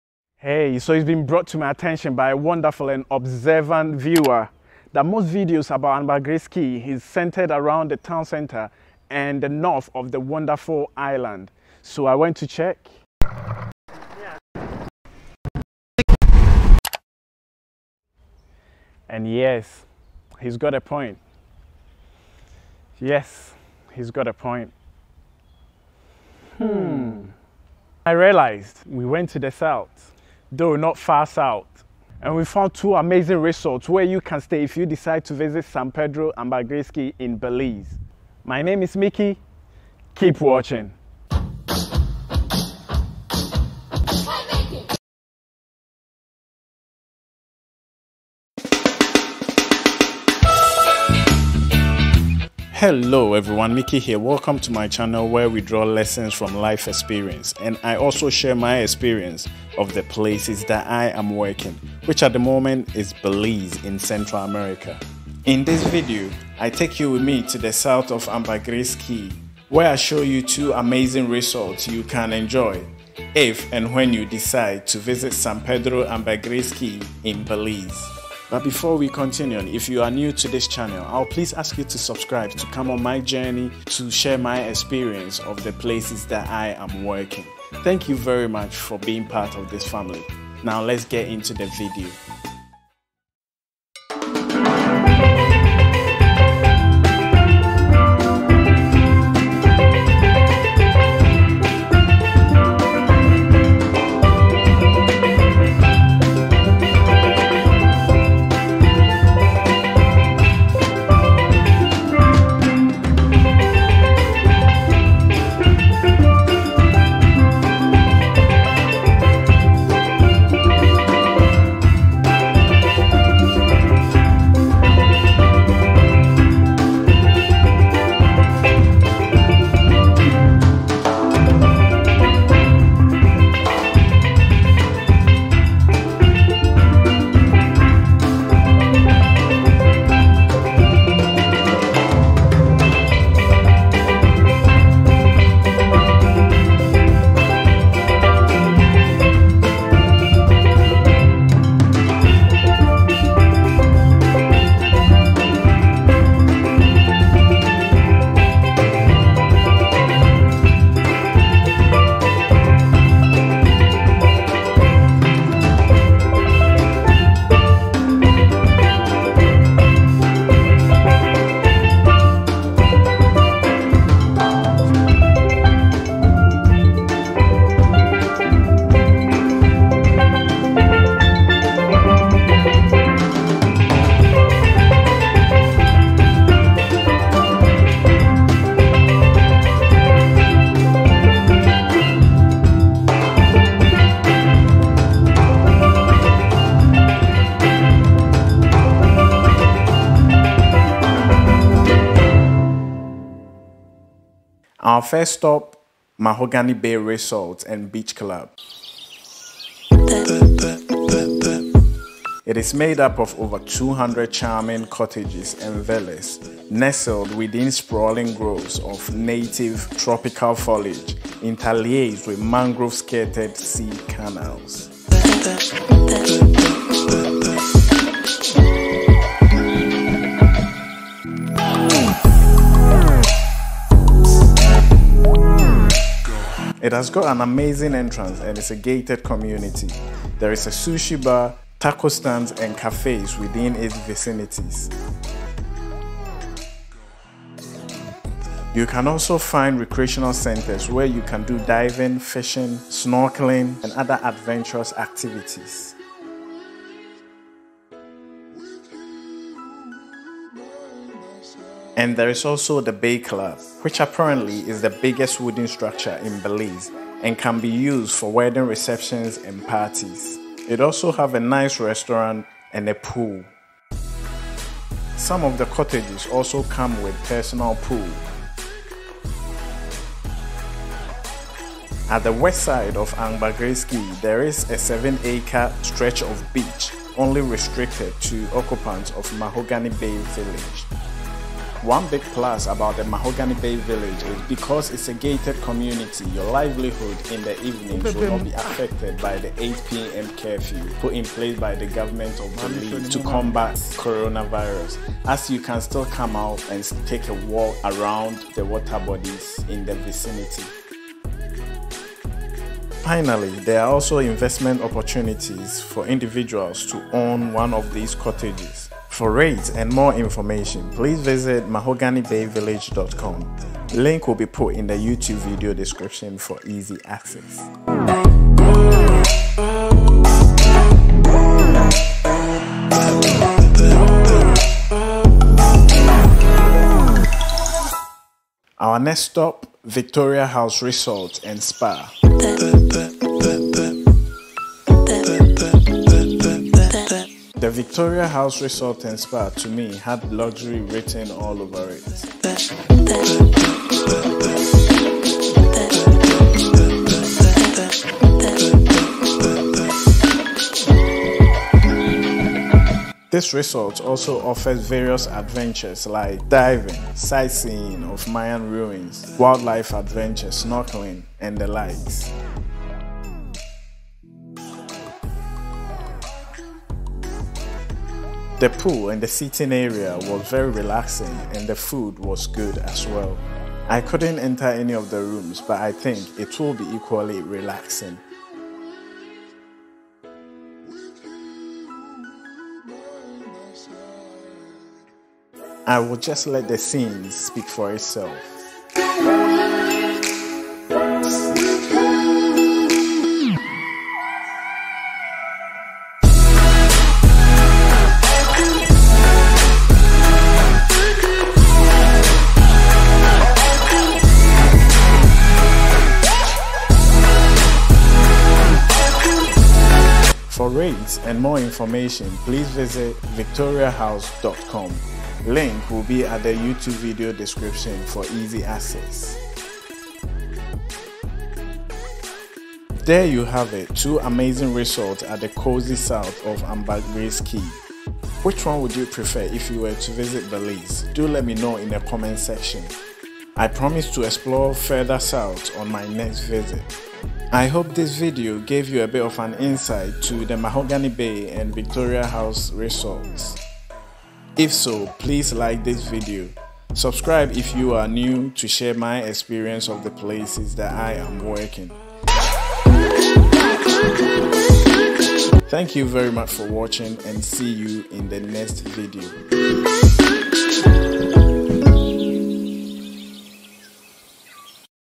San Pedro, Ambergris Caye, Belize | Mahogany Bay and Victoria House Resorts
It has been brought to my attention by a wonderful and observant viewer that most videos on Ambergris Caye is centered around the town center and the north of the wonderful Island, so I went to check. After checking, I realize he had a point.

However, I realized I went south of Ambergris Caye; though not far south and found two amazing resorts - Mahogany Bay  Resort and Beach Club, and Victoria House Resort and Spa.

In this video I take you to the outside areas of these two amazing Resorts in the south of Ambergris Caye; where you can stay in case you visit Belize.

Laptop for editing
Memory card